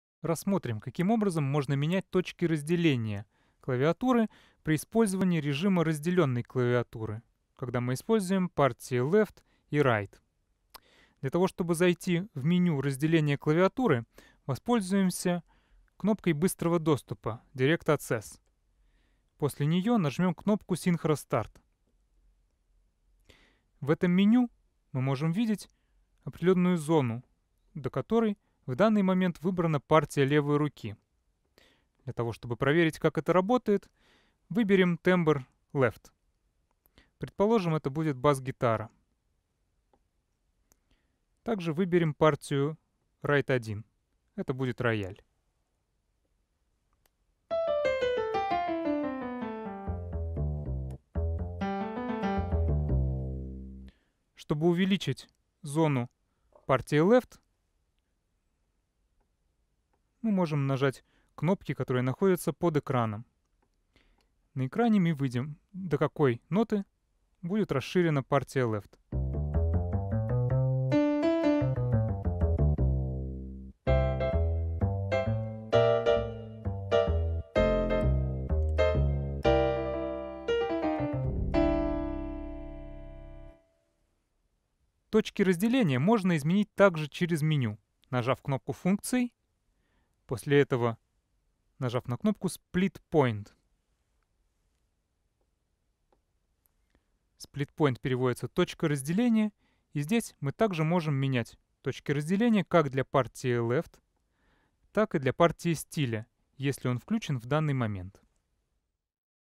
02 Настройка точек разделения клавиатуры для стиля STYLE и партии левой руки LEFT
Курс «Начинающий преподаватель» для преподавателей класса электронных клавишных инструментов под руководством Михаила Бакуменко В помощь начинающим преподавателям класса электронных клавишных инструментов компания Yamaha Music Russia представляет обновленный курс обучения основам работы с электронными клавишными инструментами Yamaha PSR-S.

Курс «Начинающий преподаватель» состоит из 19 коротких видеоуроков, доступных для просмотра. Каждый из видеоуроков посвящен отдельным аспектам работы с клавишным инструментом.